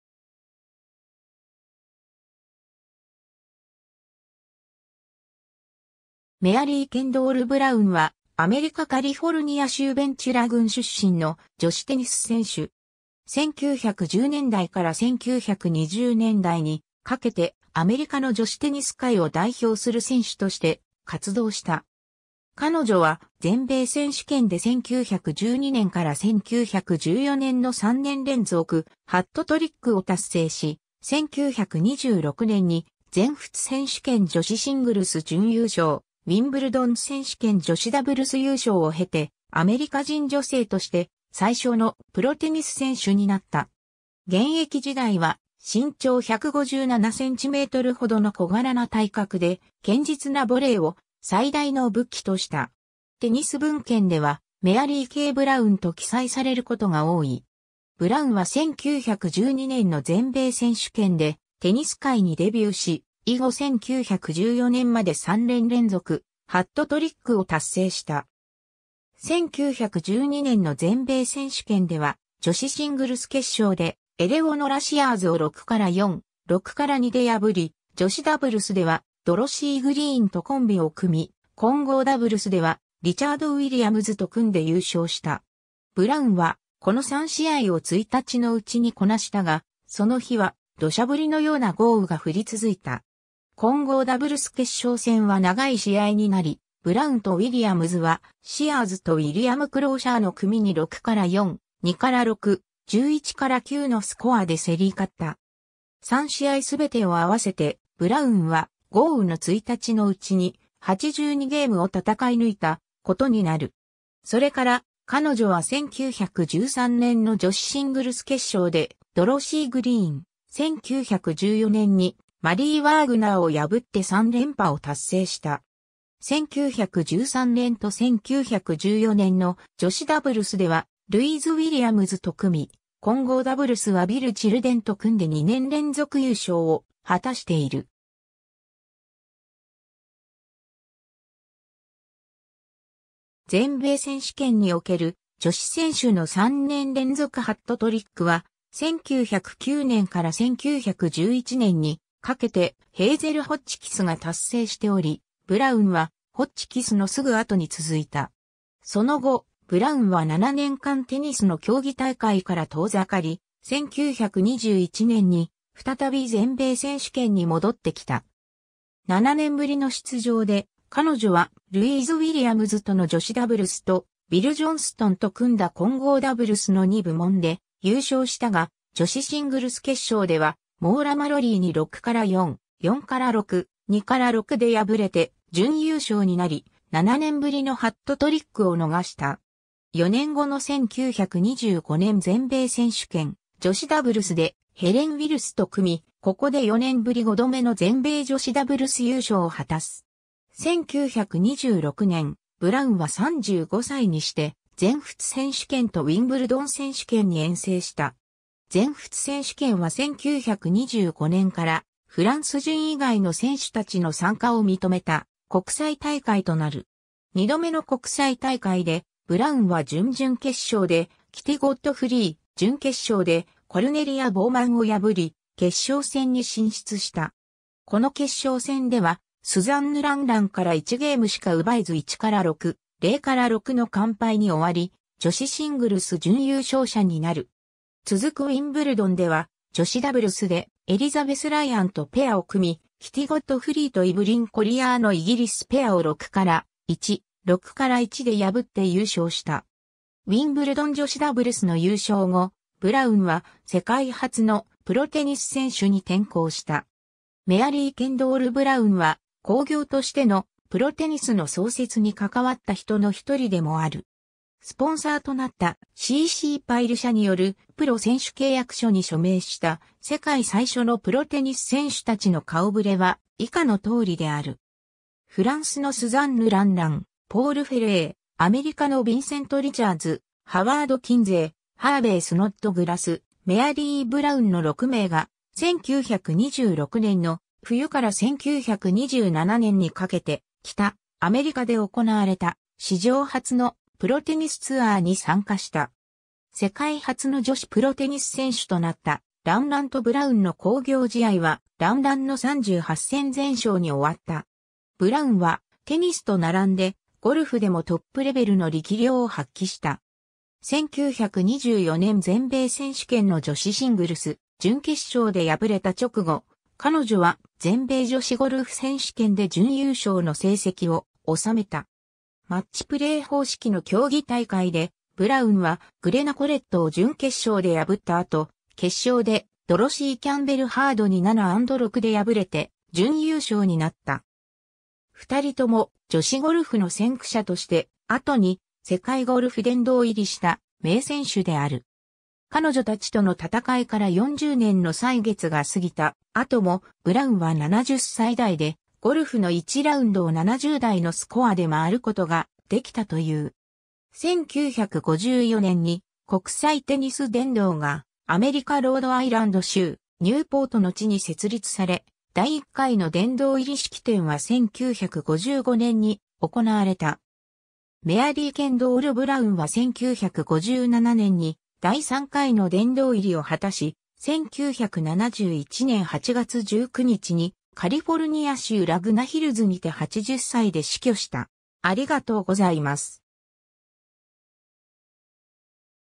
メアリー・ブラウン
メアリー・ケンドール・ブラウン（Mary Kendall Browne, 1891年6月3日 - 1971年8月19日）は、アメリカ・カリフォルニア州ベンチュラ郡出身の女子テニス選手。
1910年代から1920年代にかけて、アメリカの女子テニス界を代表する選手として活動した。
彼女は全米選手権（現在の全米オープンテニス）で1912年-1914年の3年連続「ハットトリック」（女子シングルス・女子ダブルス・混合ダブルス3部門制覇）を達成し、1926年に全仏選手権女子シングルス準優勝・ウィンブルドン選手権女子ダブルス優勝を経て、アメリカ人女性として最初の「プロテニス選手」になった。
現役時代は身長157cmほどの小柄な体格で、堅実なボレーを最大の武器とした。
テニス文献では Mary K. Browne （メアリー・K・ブラウン）と記載されることが多い。
 ブラウンは1912年の全米選手権でテニス界にデビューし、以後1914年まで3年連続「ハットトリック」を達成した。
1912年の全米選手権では、女子シングルス決勝でエレオノラ・シアーズを 6-4, 6-2 で破り、女子ダブルスではドロシー・グリーンとコンビを組み（スコア：6-2, 5-7, 6-0）、混合ダブルスではリチャード・ウィリアムズと組んで優勝した。
ブラウンはこの3試合を1日のうちにこなしたが、その日は土砂降りのような豪雨が降り続いた。
混合ダブルス決勝戦は長い試合になり、ブラウンとウィリアムズはシアーズとウィリアム・クローシャーの組に 6-4, 2-6, 11-9 のスコアで競り勝った。
3試合すべてを合わせて、ブラウンは豪雨の1日のうちに「82ゲーム」を戦い抜いたことになる。
それから、彼女は1913年の女子シングルス決勝でドロシー・グリーン、1914年にマリー・ワーグナーを破って3連覇を達成した。
1913年と1914年の女子ダブルスではルイーズ・ウィリアムズと組み、混合ダブルスはビル・チルデンと組んで2年連続優勝を果たしている。
全米選手権における女子選手の3年連続ハットトリックは、1909年から1911年にかけてヘイゼル・ホッチキス（当時は旧姓）が達成しており、ブラウンはホッチキスのすぐ後に続いた。
 その後、ブラウンは7年間テニスの競技大会から遠ざかり、1921年に再び全米選手権に戻ってきた。
7年ぶりの出場で、彼女はルイーズ・ウィリアムズとの女子ダブルスと、ビル・ジョンストンと組んだ混合ダブルスの2部門で優勝したが、女子シングルス決勝ではモーラ・マロリーに 6-4, 4-6, 2-6 で敗れて準優勝になり、7年ぶりのハットトリックを逃した。
4年後の1925年全米選手権女子ダブルスでヘレン・ウィルスと組み、ここで4年ぶり5度目の全米女子ダブルス優勝を果たす。
1926年、ブラウンは35歳にして全仏選手権とウィンブルドン選手権に遠征した。
全仏選手権は1925年から、フランス人以外の選手たちの参加を認めた国際大会となる。
2度目の国際大会で、ブラウンは準々決勝でキティ・ゴッドフリー（イギリス）、準決勝でコルネリア・ボウマン（オランダ）を破り、決勝戦に進出した。
この決勝戦ではスザンヌ・ランランから1ゲームしか奪えず 1-6, 0-6 の完敗に終わり、女子シングルス準優勝者になる。
続くウィンブルドンでは、女子ダブルスでエリザベス・ライアンとペアを組み、キティ・ゴッドフリーとイブリン・コリヤーのイギリスペアを 6-1, 6-1 で破って優勝した。
ウィンブルドン女子ダブルスの優勝後、ブラウンは世界初の「プロテニス選手」に転向した。
 メアリー・ケンドール・ブラウンは、興行としての「プロテニス」の創設に関わった人のひとりでもある。
スポンサーとなった「C・C・パイル社」によるプロ選手契約書に署名した、世界最初のプロテニス選手たちの顔触れは以下の通りである。
フランスのスザンヌ・ランラン、ポール・フェレー、アメリカのビンセント・リチャーズ、ハワード・キンゼイ、ハーベイ・スノッドグラス、メアリー・ブラウンの6名が、1926年の冬から1927年にかけて北アメリカで行われた史上初の「プロテニスツアー」に参加した。
世界初の女子プロテニス選手となったランランとブラウンの興行試合は、ランランの38戦全勝に終わった。
 ブラウンはテニスと並んで、ゴルフでもトップ...